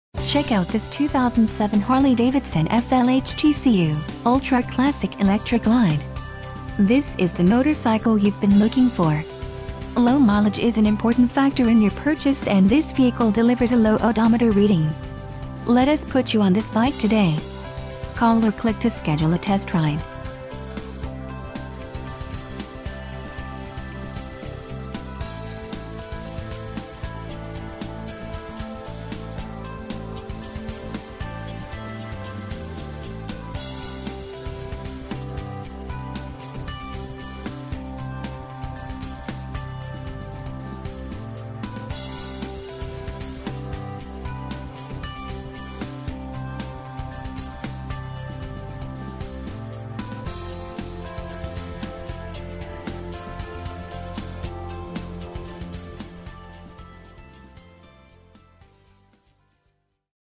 2007 Harley-Davidson FLHTCU - Ultra Classic Electra Glide...
Chopper Dave (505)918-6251 I can help you get financed today from the comfort of your home  text or call 2007 Harley-Davidson Ultra Classic Electra GlideWhen your mother made you finish everything on your plate, she was preparing you for bigger things. Open wide. The Ultra Classic is the touring smorgasbord. Spacious hardbags. Comfort-stitched seat with wraparound passenger backrest. Electronic passenger controls. Redesigned two-position King Tour-Pak for more passenger room. Hope you're still hungry. There's more. Vented fairing lowers to keep legs cool on hot days. Cruise control. CB intercom. A Harman/Kardon Advanced Audio System that clearly belts out the tunes through four speakers or headset. And for the main course: six gears of Milwaukee-brewed, 96-cubic-inch V-Twin. Make your mother proud.